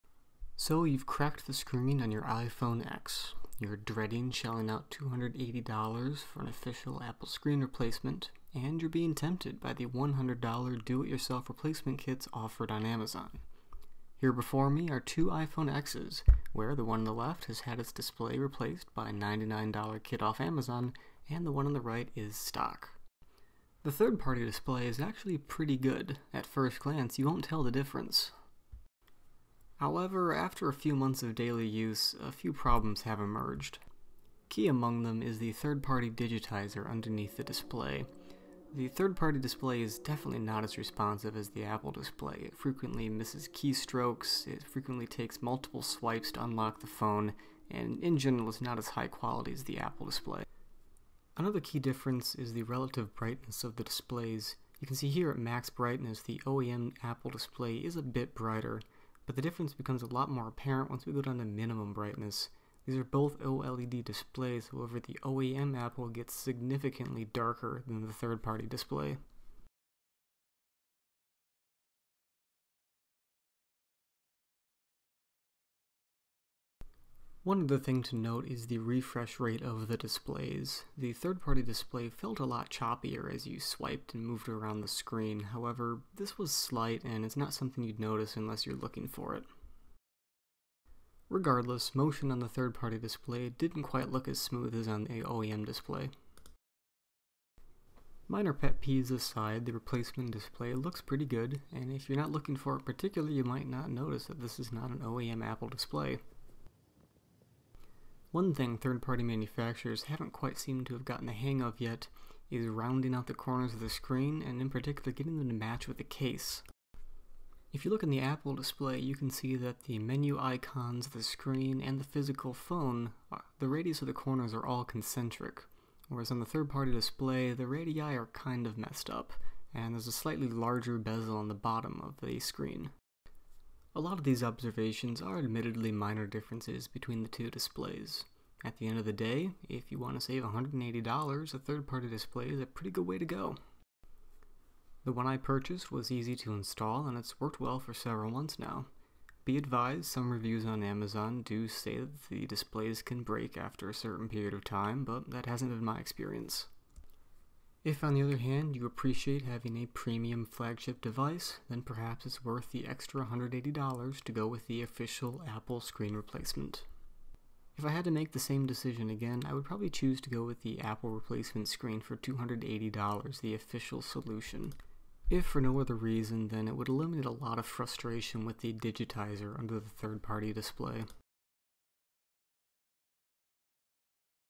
iPhone X OEM vs 3rd Party Screen Replacement
Compare the official Apple display for the iPhone X with one of the numerous "Do It Yourself" screen replacement kits you can buy off Amazon for around $100.